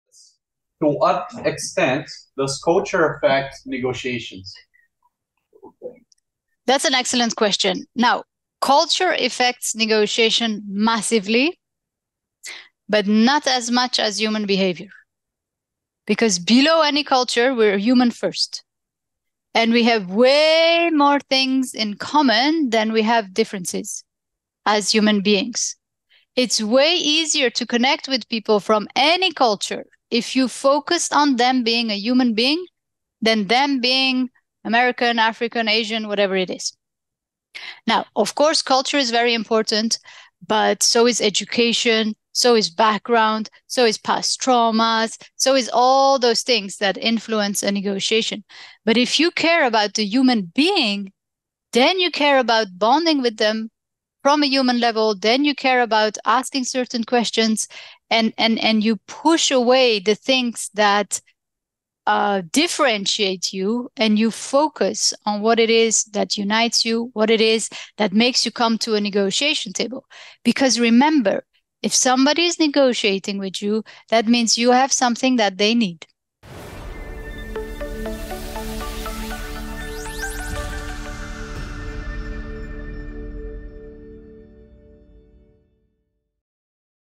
Overcoming Cultural Differences in Negotiations | Business School Lausanne With Lousin Mehrabi
How to deal with cultural differences in negotiations? 🌏

You won't believe this but we have 8 different cultures in our household!

I work with clients worldwide and have lived in 4 countries.
On any given day I deal with at least 5-10 different nationalities.

So how to do this successfully?

Of course, we can go deep into all the different cultural codes.
But you know what I've learned dealing with so many cultures for 40 years?

That the key to successfully negotiating with people from other cultures is to remember this:

Behind the countries we're from,
The languages that we speak,
Our skin color or ethnicity...

𝗪𝗲 𝗮𝗿𝗲 𝗮𝗹𝗹 𝗵𝘂𝗺𝗮𝗻 𝗯𝗲𝗶𝗻𝗴𝘀

That means we have more similarities than differences.

Culture has a strong influence on us,

But so do education, background, upbringing, past trauma, and everything else that people bring to the negotiation table with them!

So the best way to successfully negotiate with people from diverse cultural backgrounds is to:

1️⃣ See the human behind their culture
2️⃣ Focus on finding the common ground - what you would both benefit from if you were to come to an agreement
3️⃣ Look beyond what differentiates you from them, including culture
4️⃣ Find what brings you together and how you can create value

Of course, as a form of respect, you want to learn a bit about their culture.
Mainly not to make big cultural mistakes.

But giving too much attention to the culture won't get you to a deal -
unless you incorporate true negotiation skills to reach an agreement.

With integrity and ethics.

Thoughts?

Love & Motivation,
Lousin Mehrabi